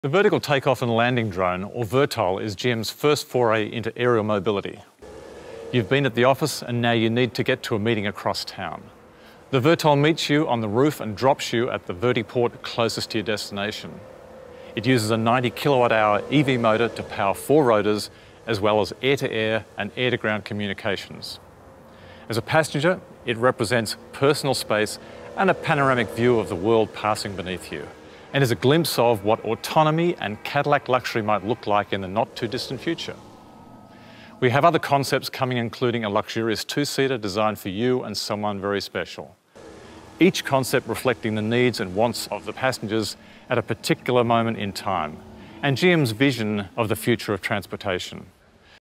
General Motors reveals futuristic flying Cadillac car
General Motors on Tuesday presented a futuristic flying car  - a self-driving Cadillac which takes off and lands vertically and carries the passenger above the streets and through the air.